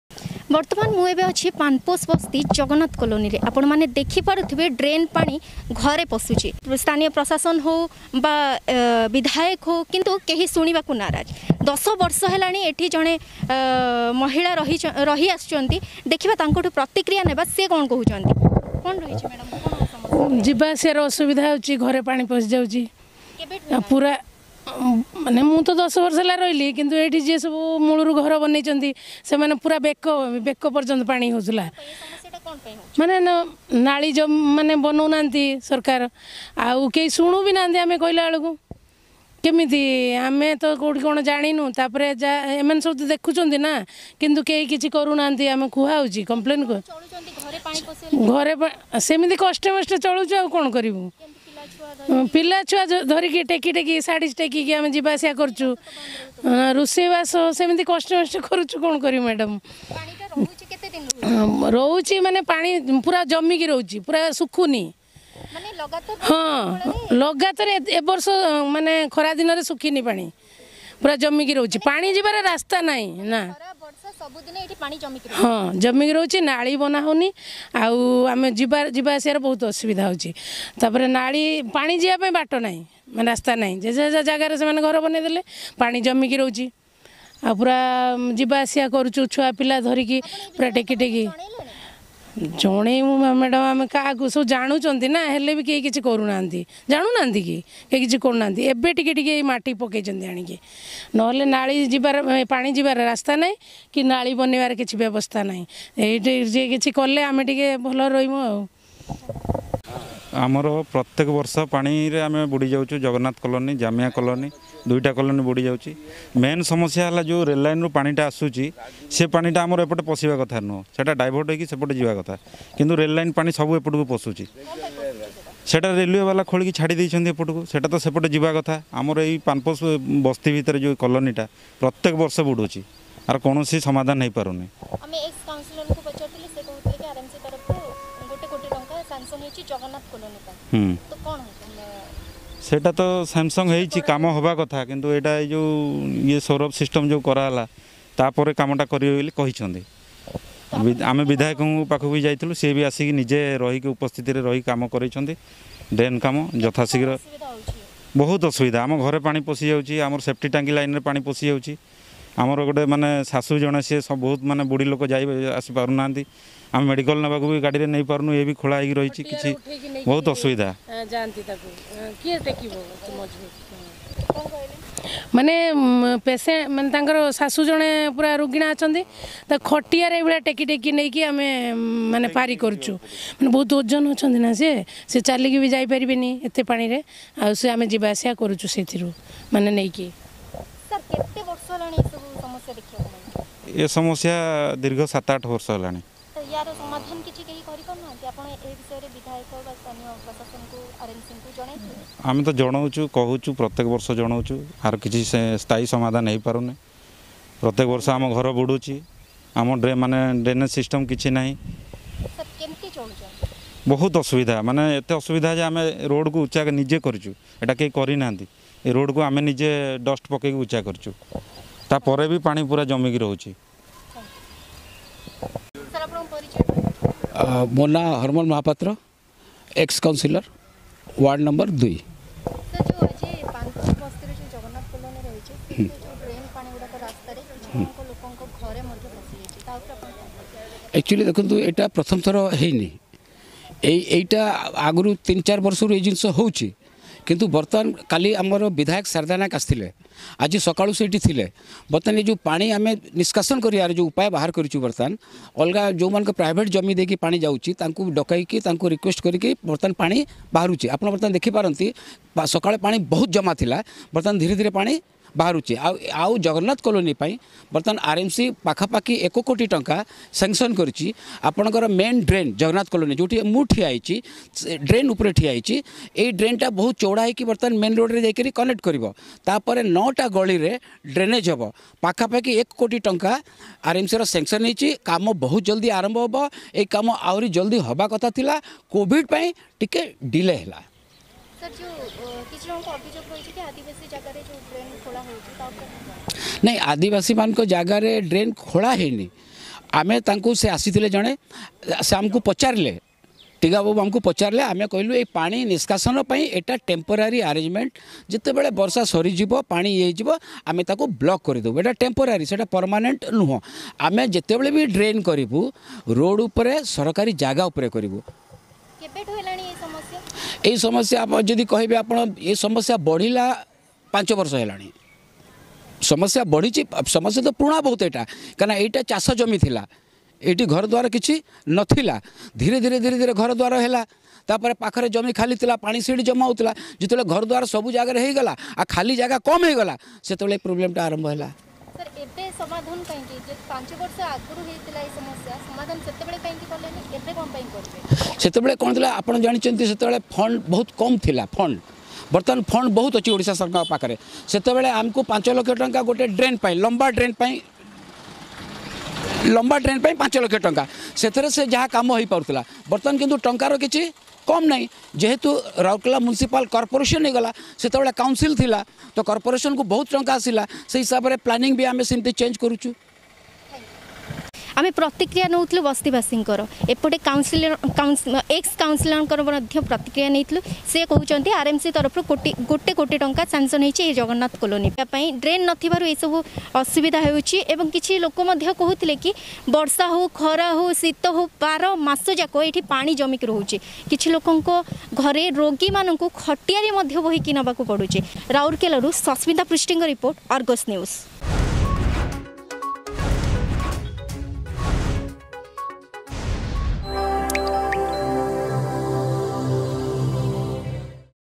ରାଉରକେଲା ପାନପୋଷ ବସ୍ତିରେ ପଶିଲା ନାଳ ପାଣି
ରାଉରକେଲା ପାନପୋଷ ବସ୍ତି ଅଞ୍ଚଳର ଜଗନ୍ନାଥ କଲୋନୀରେ ଘରେ ଏବଂ ରାସ୍ତା ରେ ନାଳ ପାଣି ଜମି ରହିଛି ।ସ୍ଥାନୀୟ ପ୍ରଶାସନ ଆସି ଖାଲି ଦେଖିକି ଯାଉଛନ୍ତି କିନ୍ତୁ ଏହାର ସ୍ଥାୟୀ ସମାଧାନ କରୁ ନଥିବାର ଅଭିଯୋଗ କରିଥିଲେ